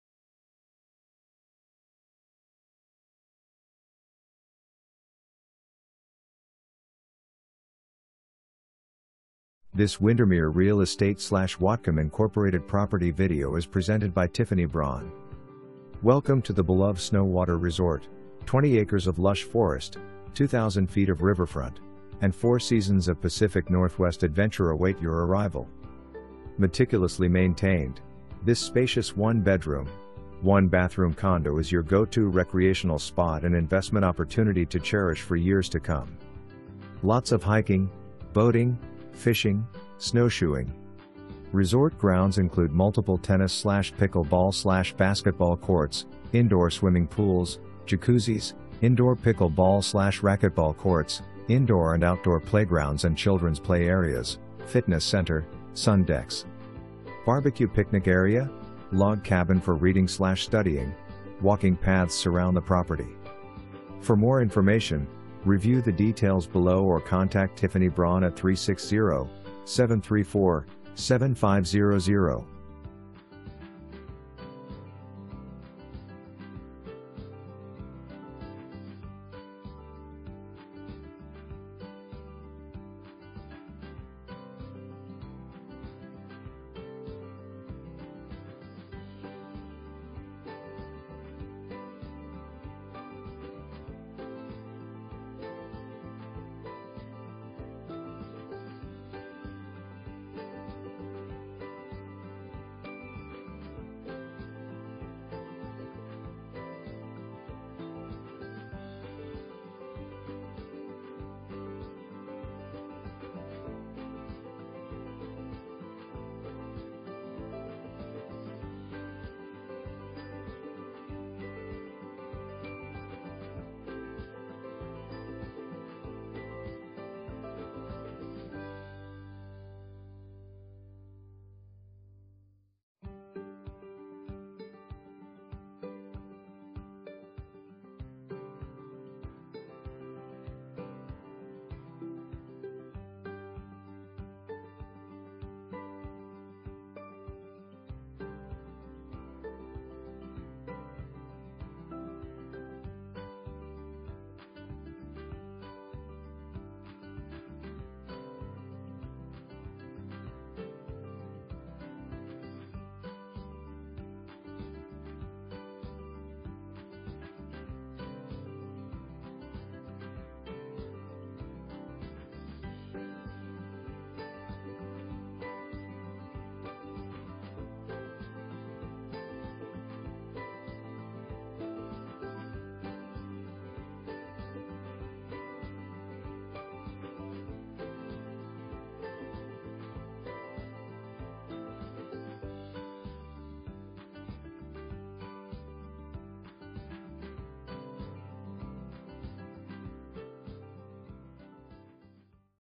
Windermere Real Estate/Whatcom, Inc. - 10500 Mt Baker Hwy 207
10500 Mt Baker Hwy 207
Deming, WA 98244

Welcome to the beloved Snowater resort - 20 acres of lush forest, 2000 feet of riverfront, and 4 seasons of Pacific Northwest adventure await your arrival! Meticulously maintained, this spacious one bedroom, one bathroom condo is your go-to recreational spot and investment opportunity to cherish for years to come! Lots of hiking, boating, fishing, snowshoeing. Resort grounds include multiple tennis/pickle ball/basketball courts, indoor swimming pools, jacuzzis, indoor pickle ball/racketball courts, indoor and outdoor playgrounds and children's play areas, fitness center, sun decks. BBQ picnic area, Log Cabin for reading/studying, walking paths surround the property.

1 full bath

Tiffany Braun
Windermere Real Estate/Whatcom, Inc.